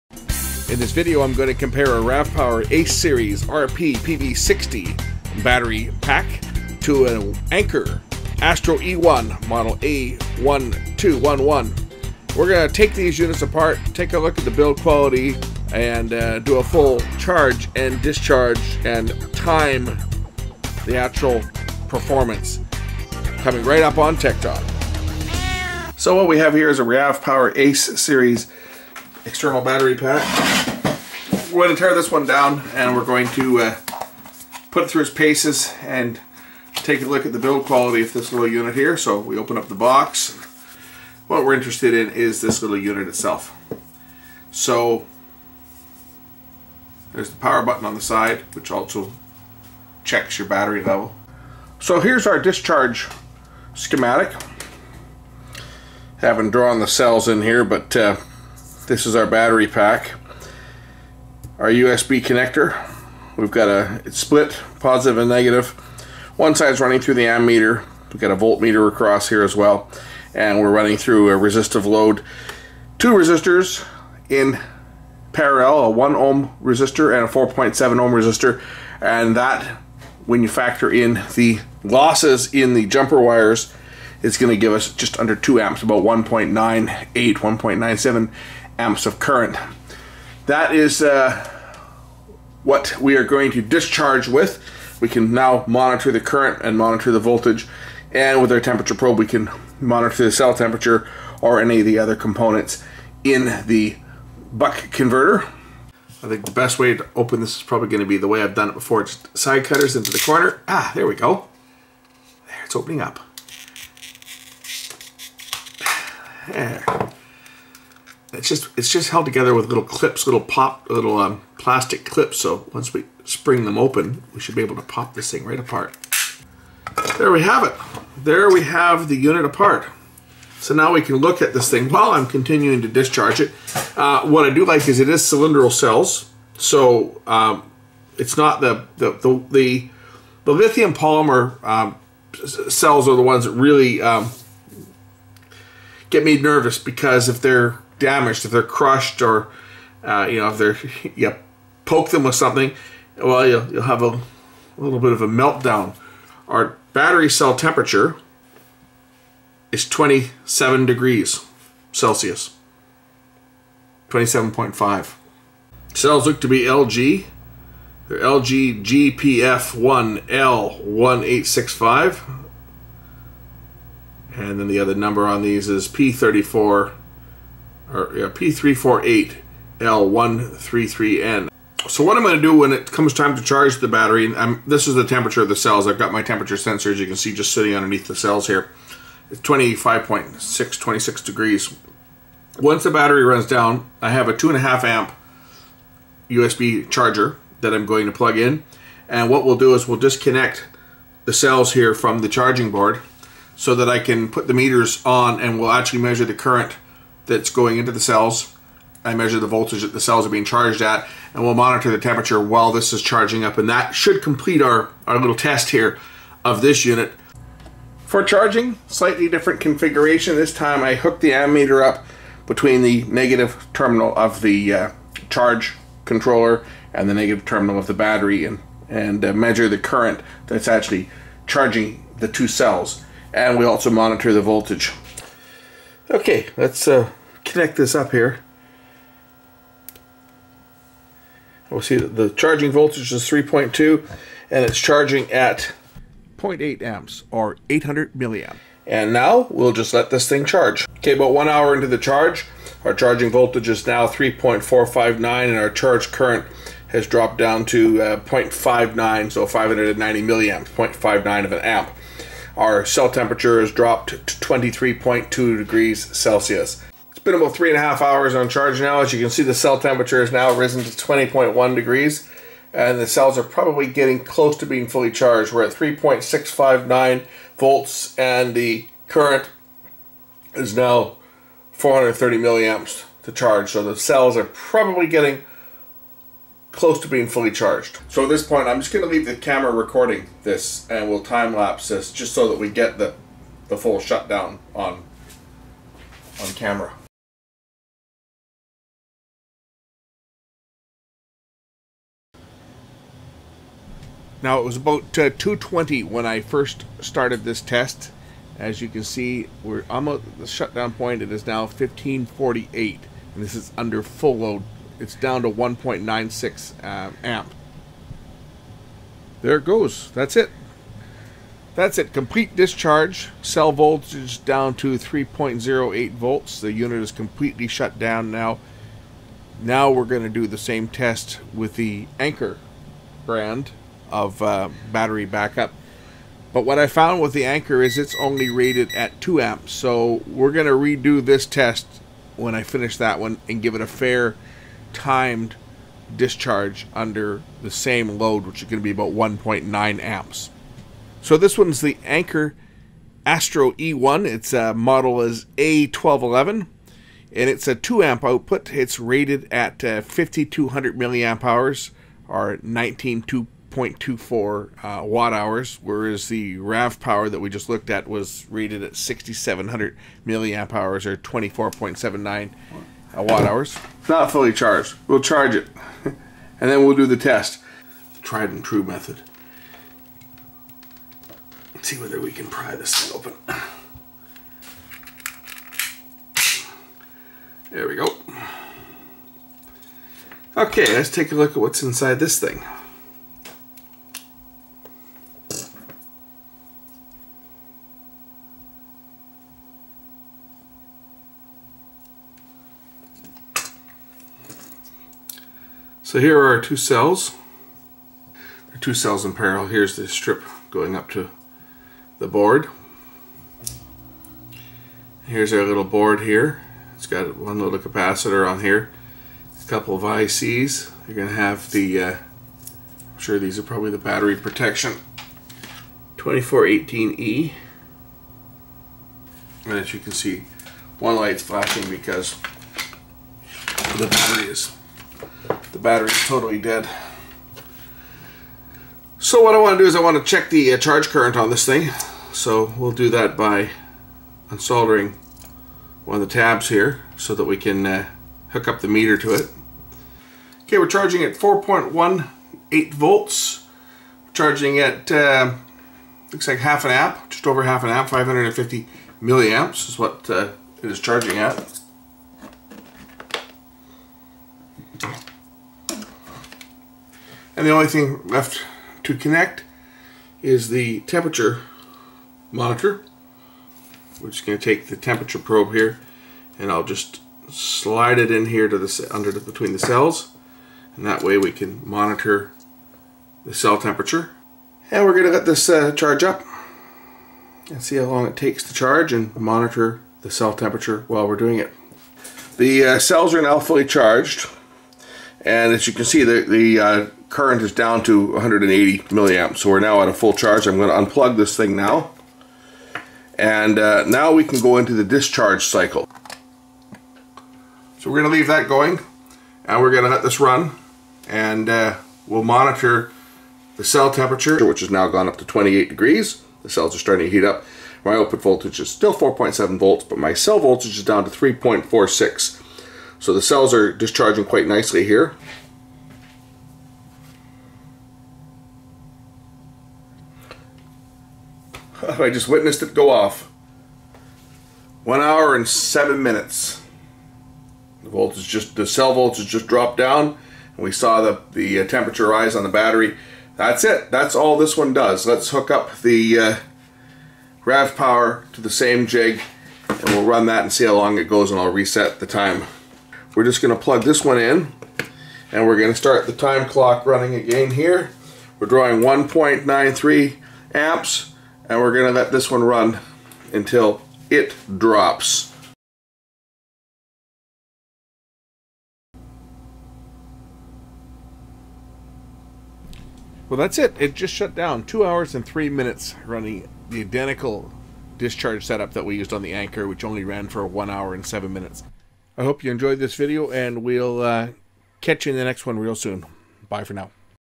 Ravpower Ace RP PB60 vs Anker Astro A1211 charge discharge
2 popular power banks are torn down and put through charge discharge test cycles.